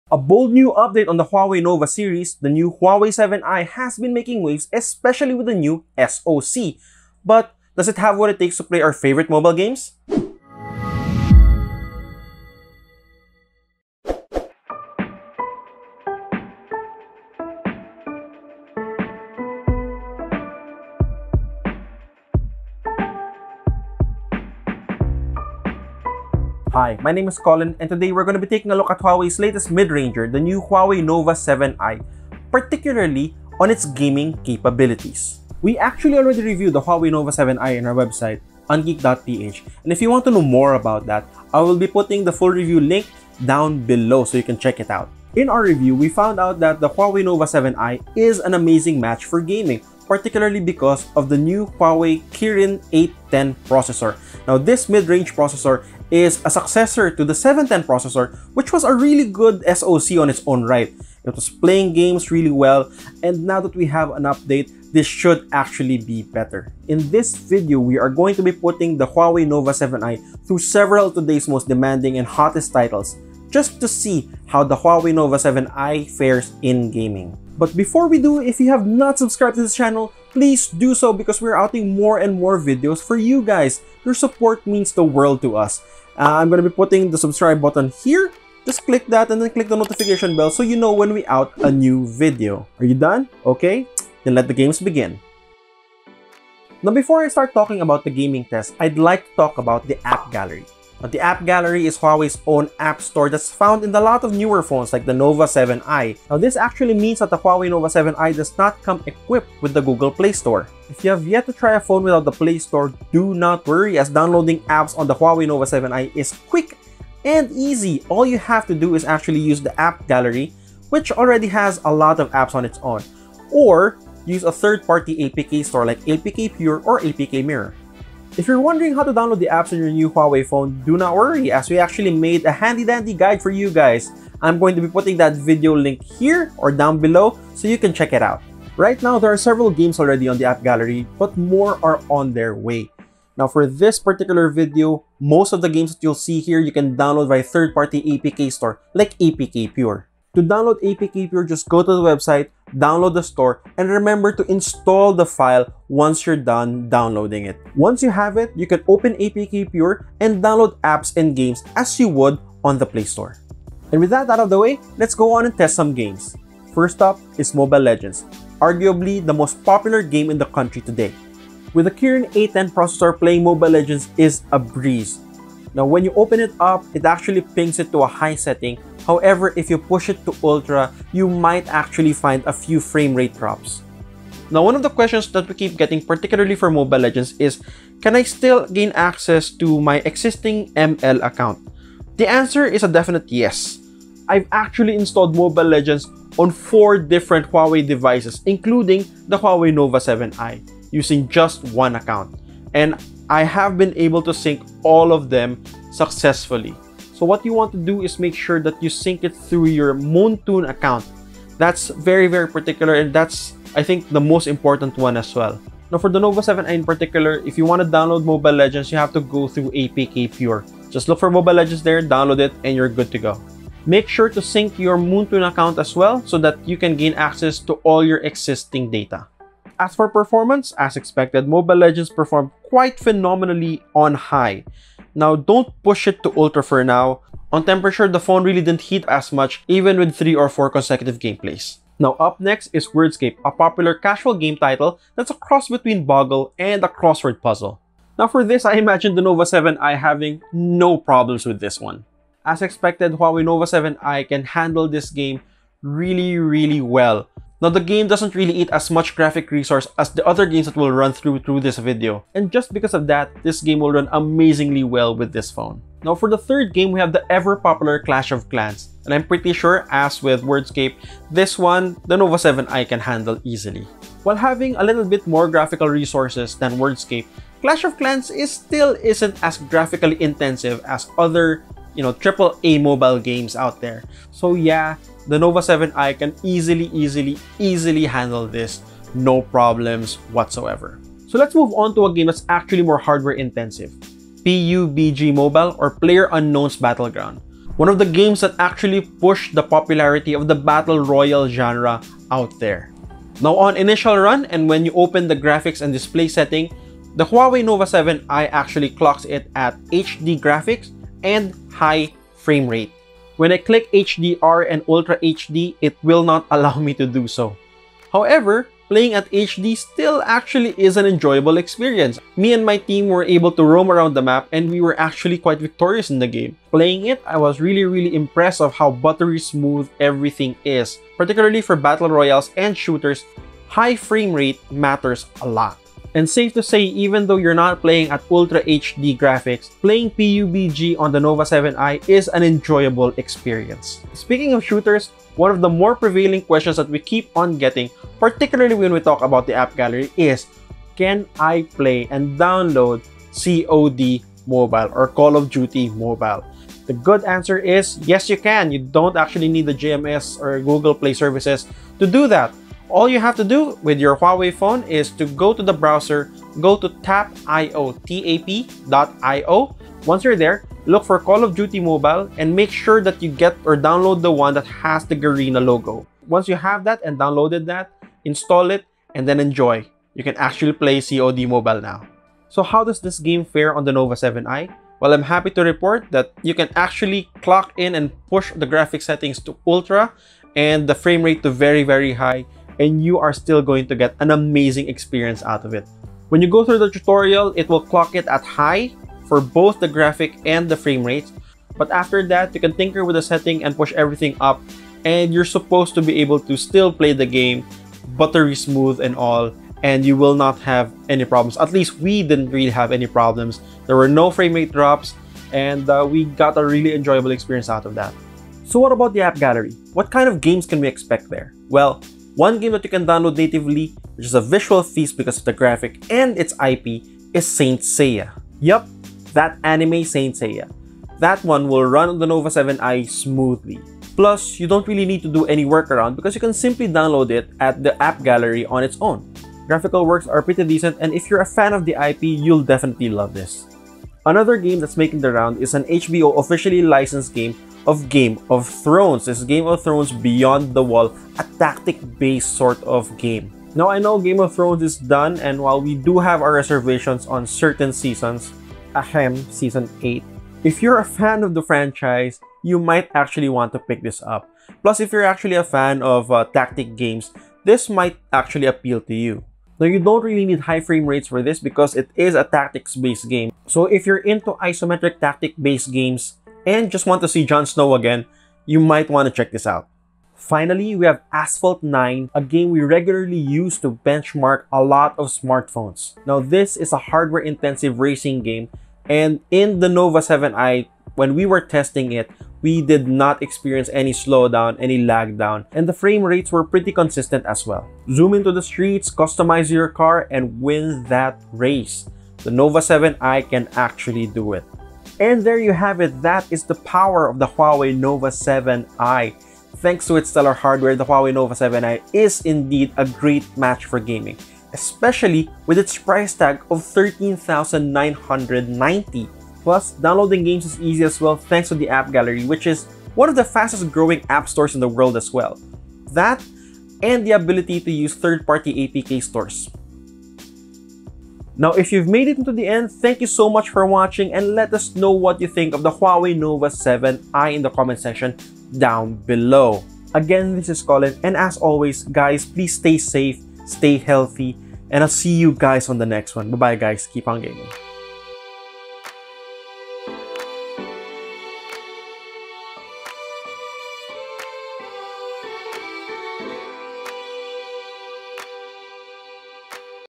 Huawei Nova 7i Gaming Test | Play Everything?
Huawei's mid-ranger has been out for quite a while now but it still packs quite a punch. In this video we test the smartphone out on today's most demanding and most downloaded mobile games (and then some) just to see if it can keep up with today's mobile gamers.

If you want to know how to download various apps without using the Google Play Services we have a step-by-step guide